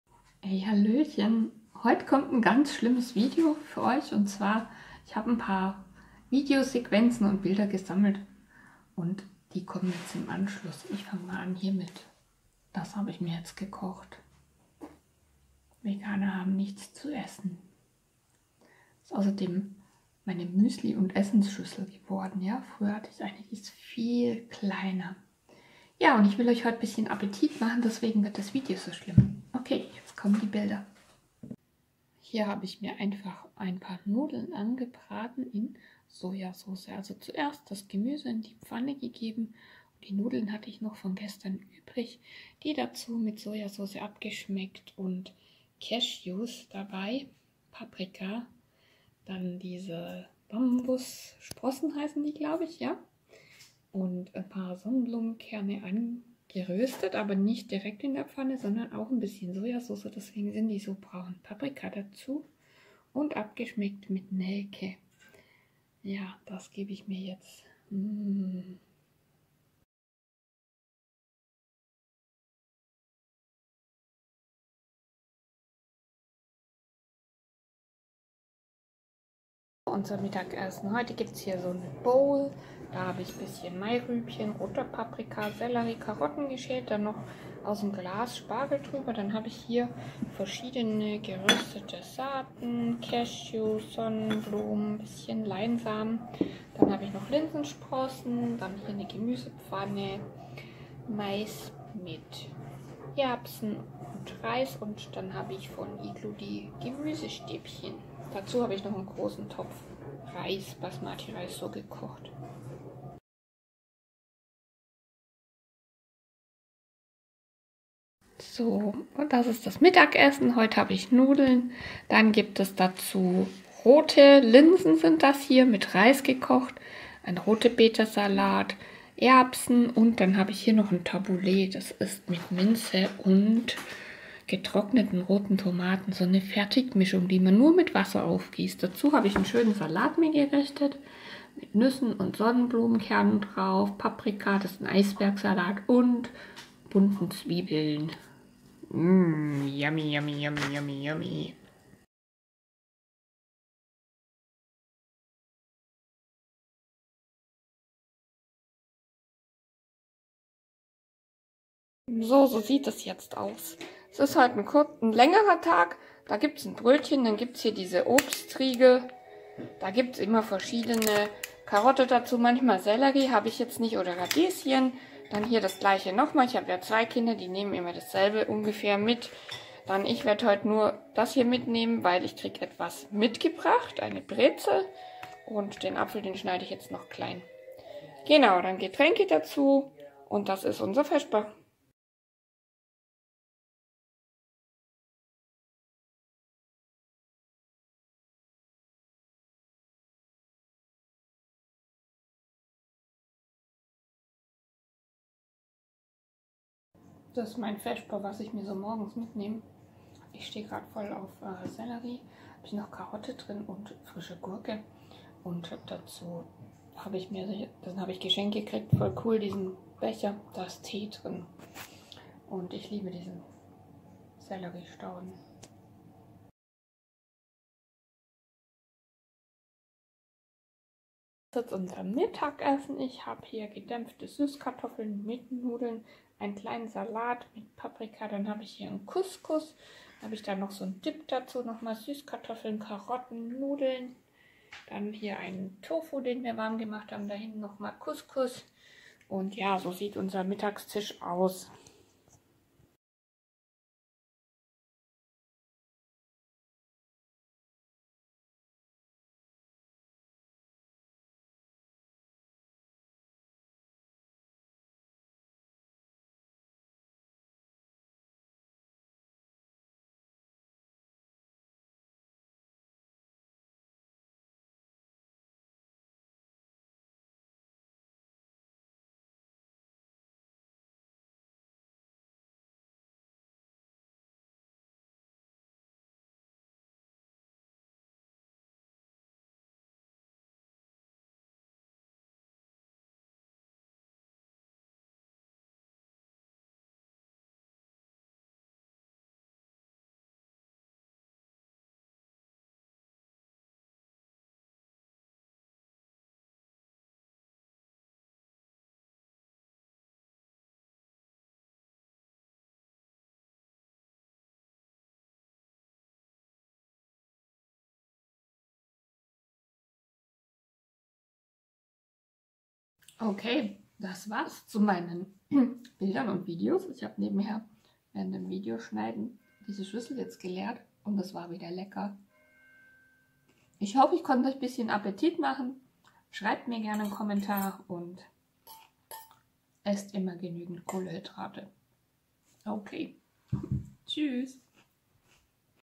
Food VLOG - was esse ich
Ein paar kurze Videos und Bilder - so esse ich.